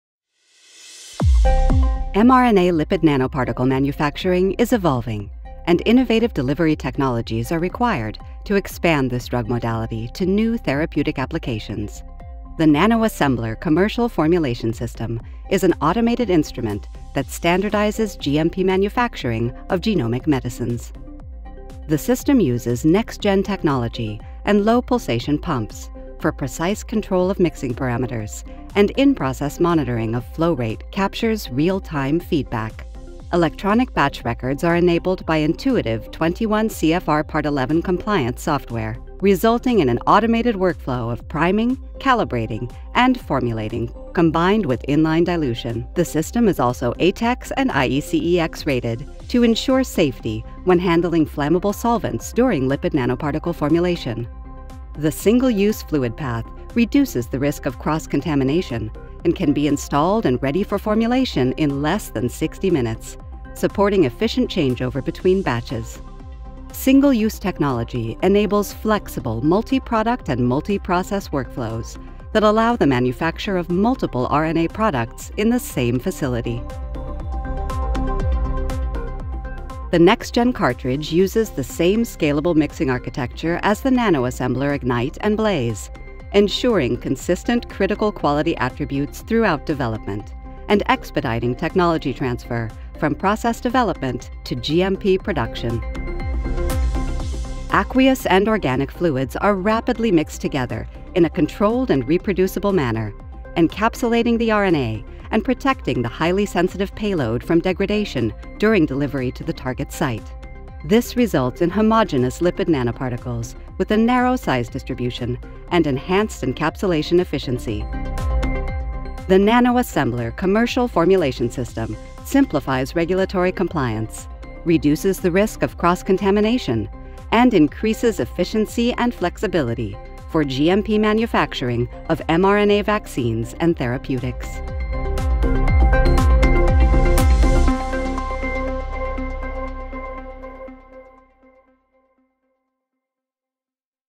NanoAssemblr Commercial Formulation System Overview
Learn how the NanoAssemblr® commercial formulation system enables efficient changeover and robust manufacturing processes for the clinical and commercial production of lipid nanoparticles under cGMP conditions.